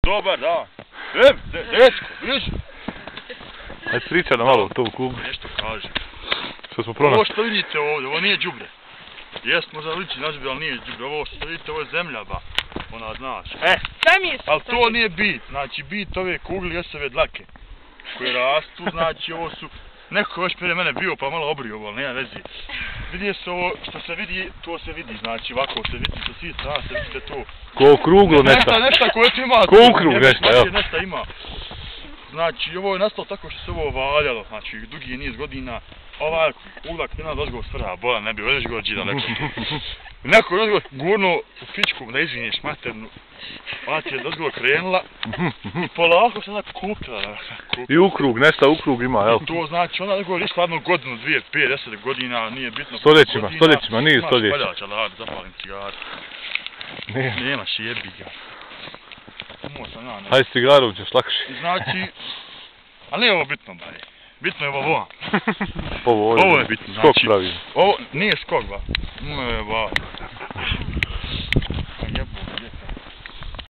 Ermin objasnjava nastanak kugle koja se pojavila sasvim slucajno u predjelu Barice(Piromani), poznato skijaliste u Podvincima..